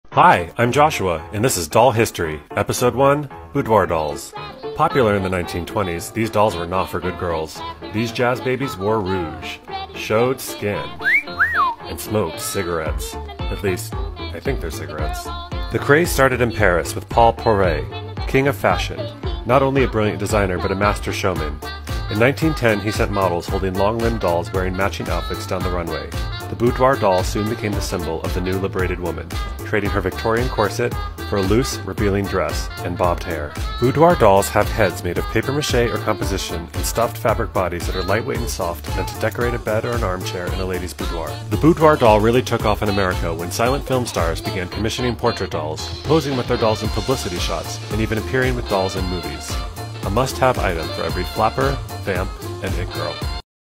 Are these the "Bad Girls" of dolls?? 😈 Doll History Part 1: Boudoir Dolls
Let me know if you'd like to see more doll history!